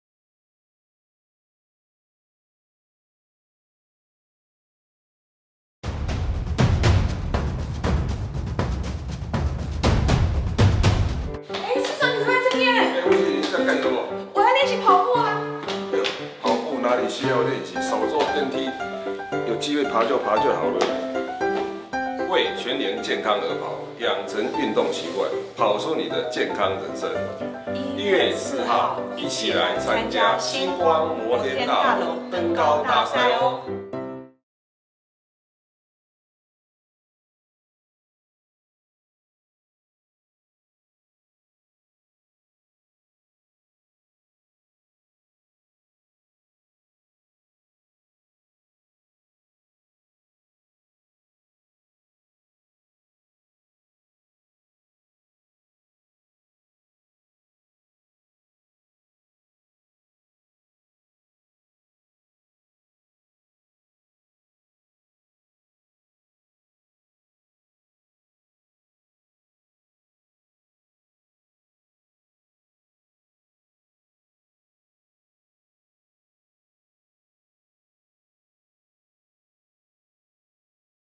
1090104 阿北與007 新光登高賽 宣傳短片（柯文哲 吳欣盈）
影片來源：吳欣盈臉書

【出席新光大樓登高賽 柯文哲盼推廣全民運動的理念】

臺北市長柯文哲4日早上出席「第39屆新光人壽保險摩天大樓登高大賽暨救護車捐贈儀式」，柯文哲致詞時表示推廣全民運動的理念，今年是第39屆，過去每年都會舉辦，但今年特別的是新光人壽捐贈本府救護車。
柯文哲表示臺北市政府的救護車都是來自民間的捐獻，去年統計資料，臺北市平均有13萬趟緊急救護勤務，比較特別的是有121位，到院前心跳停止的民眾，因為救護車移動迅速抵達醫院，再加上臺灣的葉克膜技術是全世界最發達的，經急救後成功的將患者救回，讓許多家庭免於破碎，並節省了難以計數的社會資源支出。
柯文哲表示政府預算有限，民間力量無窮，在這要感謝過去多年來捐獻救護車給臺北市政府的單位及個人，也因為他們的捐贈，讓臺北市的救護系統更完善。
柯文哲提到今天新光人壽所捐贈的救護車，會編列在消防局的後港分隊，造福士林地區的民眾，在這裡也特別感謝，柯文哲表示消防局的同仁，也會以取之於社會、用之於社會的精神，來救助更多的臺北市民，最後，謝謝大家今天的盛情參與，也祝福大家身體健康，萬事如意！